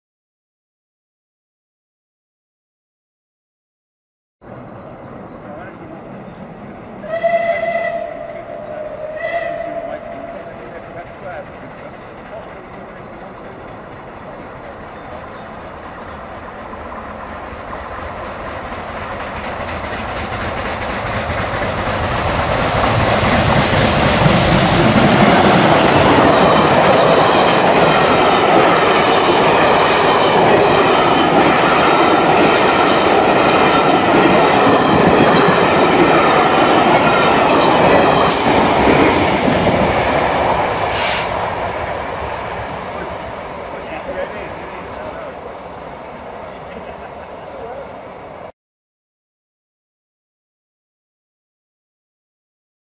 Steam Train: 35028 "Clan Line" on VSOE British Pullman, 13 Jun 2012, Ascot
This is a video of preserved ex-Southern Railway "Merchant Navy" class Bulleid pacific loco no 35028 "Clan Line".

She is seen on 13th June 2012 pulling the Orient Express "British Pullman" steam excursion from London to Bristol. The location is a few hundred yards after Ascot Station, on the Ascot to Reading line.

Built in 1948, Clan Line is owned by the Merchant Navy Locomotive Preservation Society, who purchased her from British Railways for £3850 in 1967. She returned to mainline service in 1974, and has had a longer career in preservation than under BR ownership!

The weather for this trip was lovely and sunny, the first time we had seen the sun for a couple of weeks!